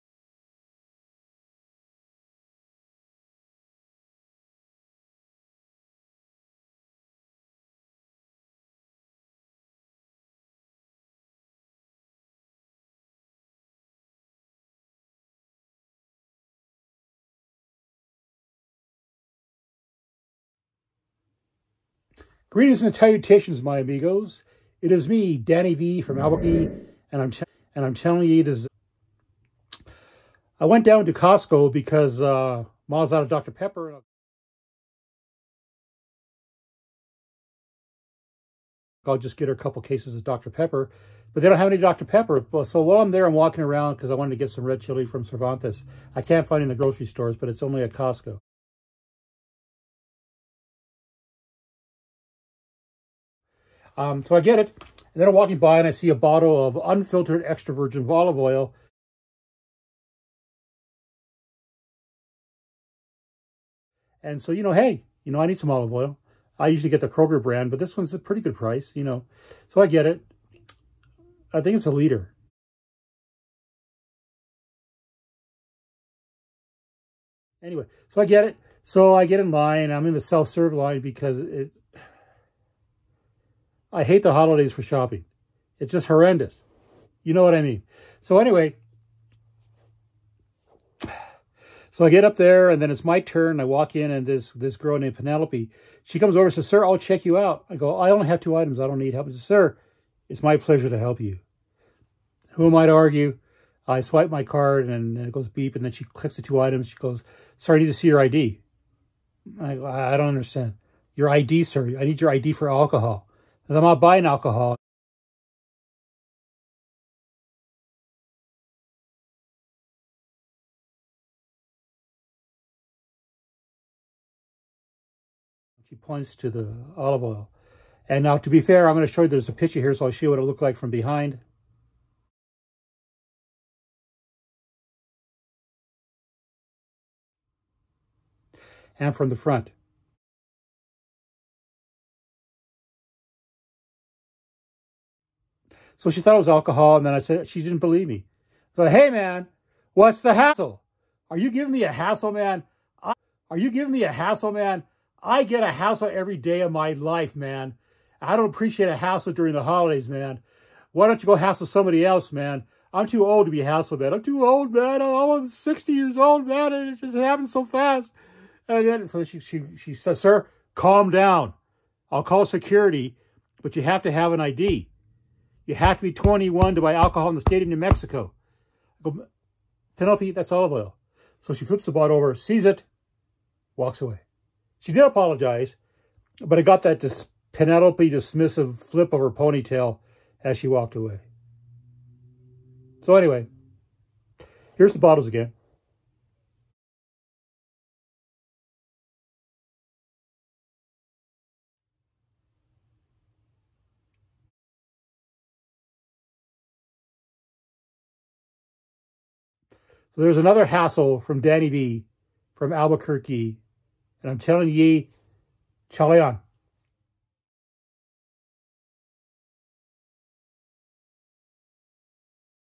Un Film De Danny V from Albuquerque #1
A trip to  Cosco yields a Hassle for Danny V